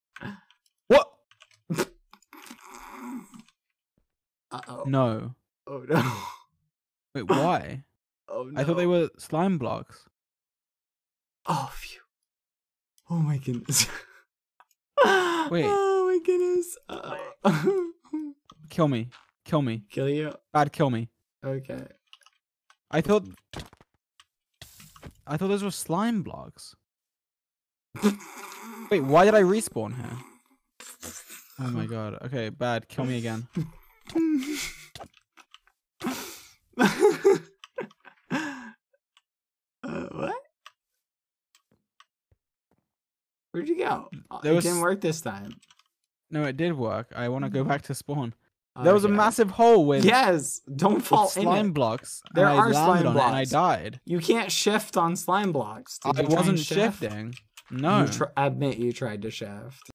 GeorgeNotFound's Repeated Deaths
6/13/2021

George kept dying in this one spot over & over. XD